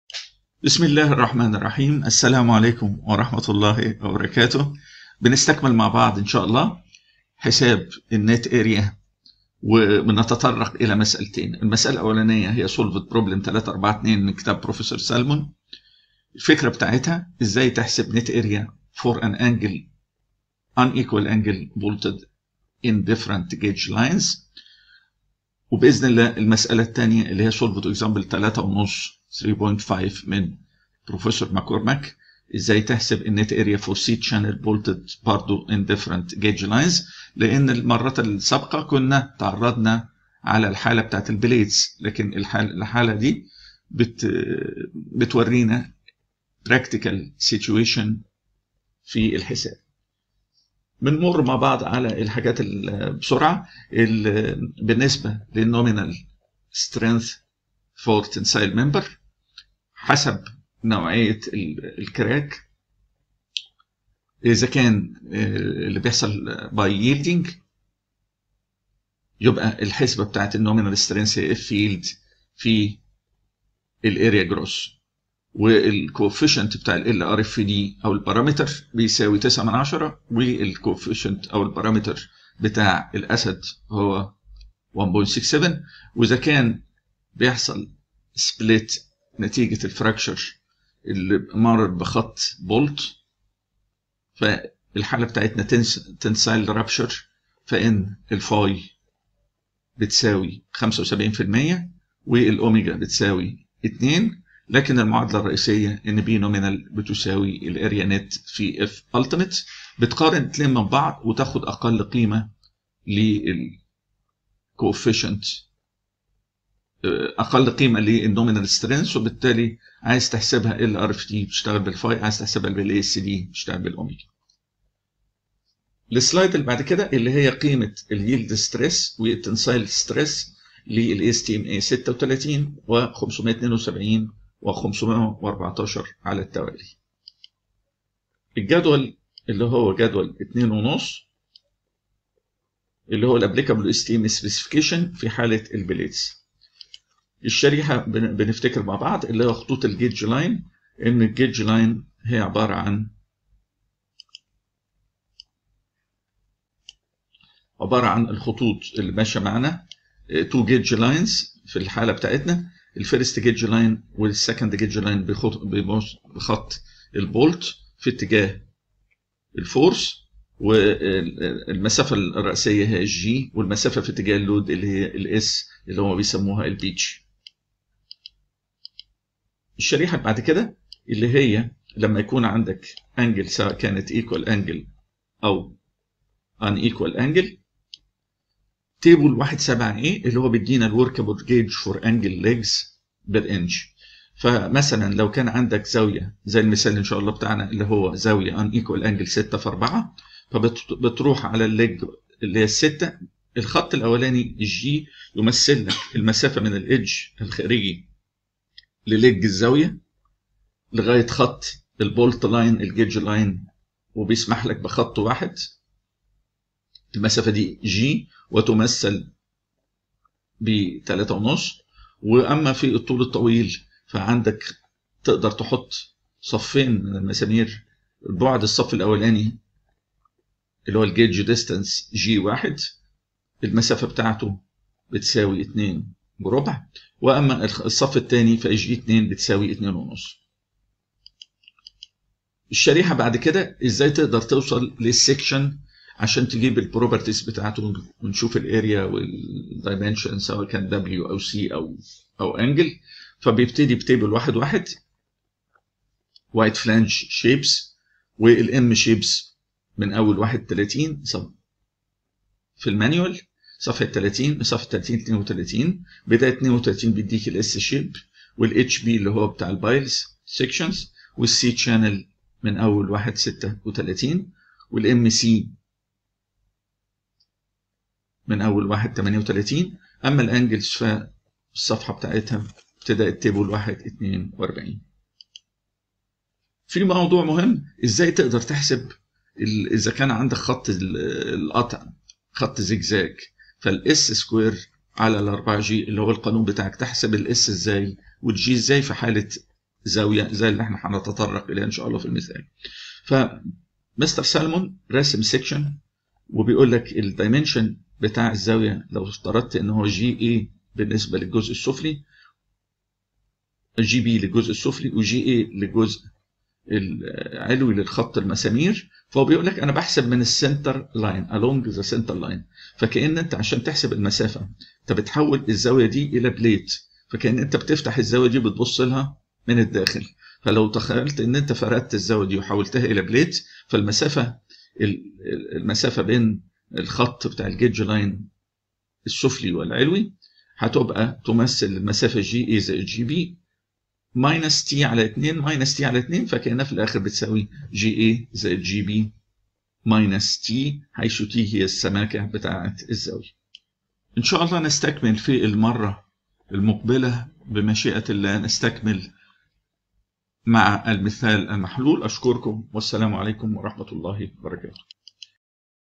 Civil 120-78-Net area for an unequal angle with several gauge lines-F E Exam Review -Arabic.
Net area for an equal angle with several gauge lines, how to estimate the gauge line distances for an angle, the final g distance for an angle after considering the thickness of the angle.
This video has a closed caption.
Net area for an equal angle with several gauge lines.
tension members
قائمة التشغيل لفيديوهات عناصر الشد فقط
تحميل ملفات الشرح فى صورة بى دى اف من الرابط